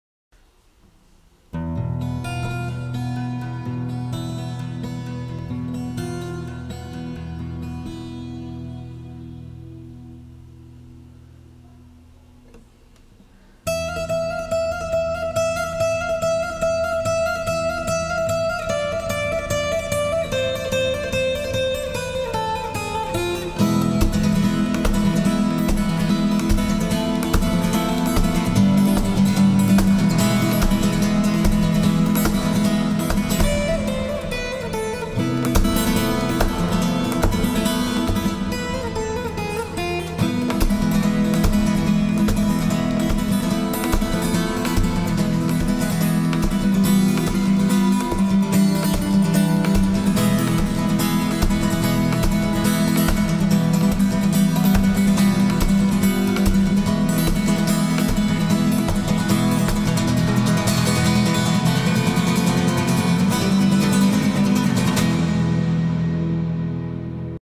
Beats of Guitar... Free style guitar || saiprashob
Free style guitar . Own composition . Play as you like moment .
Pls do listen with headphones for clarity .
It's a live recording..so forgive the flaws if any.